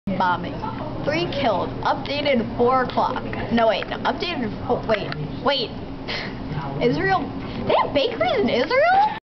Bakeries in Israel?
some idiocy during study hall starring the love of my life, Natalie!